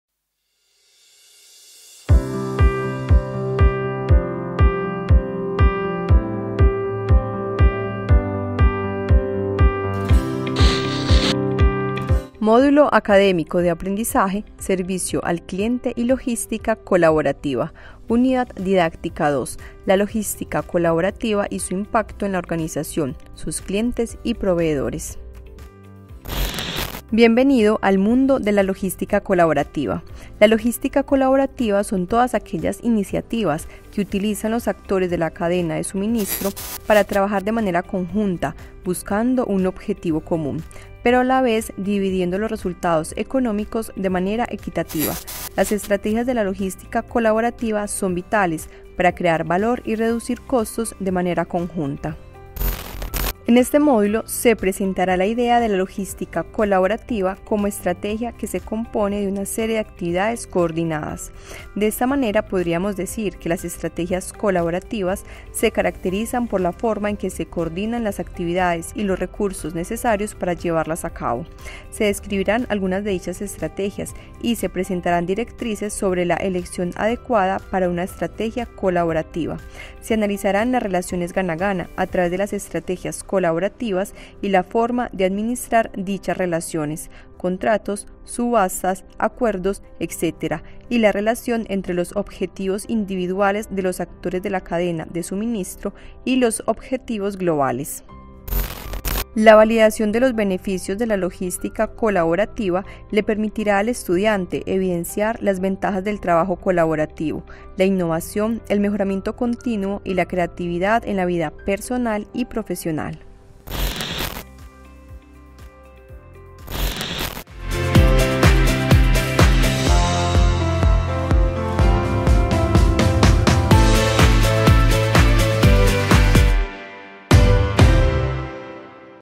Servicio al cliente y logística colaborativa- UD2
Servicio al cliente y logística colaborativa- UD2 - La logística colaborativa y su impacto en la organización, sus clientes y proveedores.

Bienvenido al mundo de la logística colaborativa. La logística colaborativa son todas aquellas iniciativas que utilizan los actores de la cadena de suministro para trabajar de manera conjunta buscando un objetivo común, pero a la vez dividiendo los resultados económicos de manera equitativa. Las estrategias de logística colaborativa son vitales para crear valor y reducir costos de manera conjunta.

Medellín, Colombia